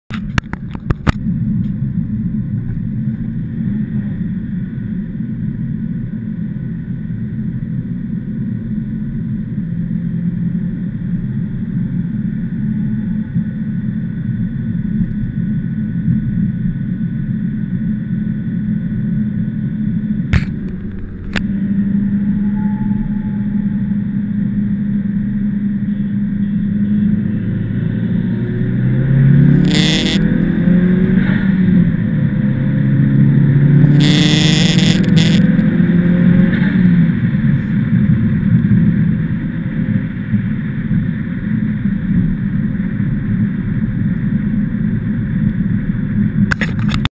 WRX vs WRX Wagon
two of my buddies going head to head in the biggest battle of the year. Been setting this race up for decades..

camera car: evo8